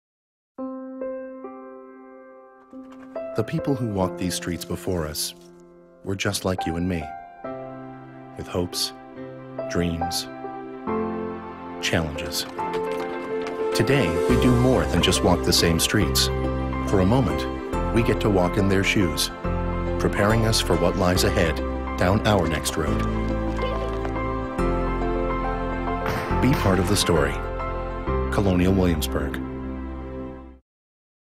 Be Part of the Story - Streets
TV spot for Colonial Williamsburg - Produced by McCann Erickson
VO: Dave B. Mitchell